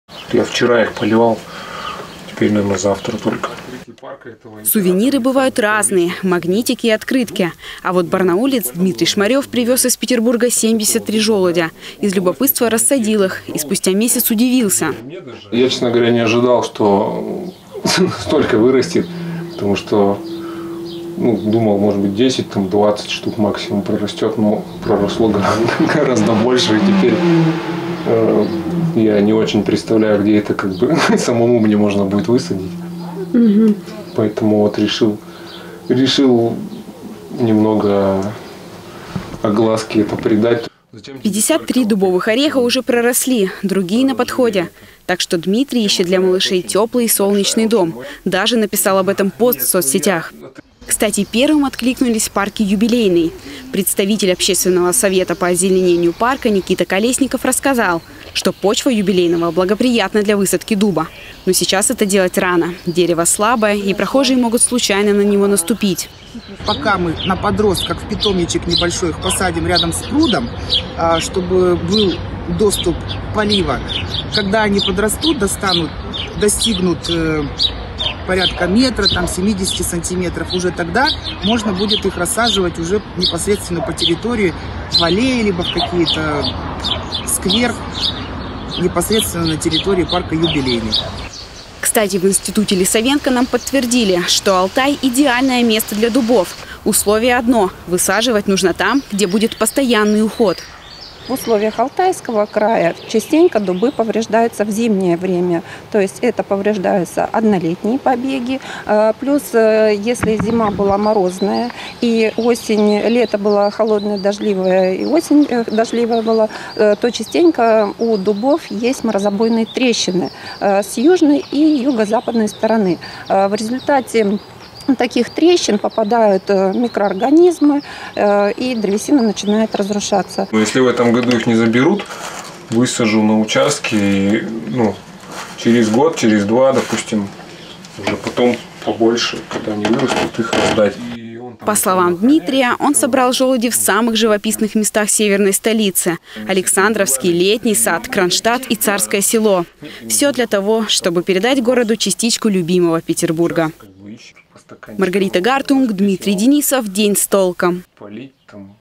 Барнаулец привёз 53 жёлудя из Санкт-Петербурга и вырастил дубы
Житель Барнаула ищет дом для целой рощи маленьких дубков, которую он вырастил – сам. Не так давно Дмитрий Шмарёв привез домой 53 желудя из Санкт-Петербурга. И удивился, когда они ВСЕ – взошли. Что молодой человек планирует делать с таким «молодняком»?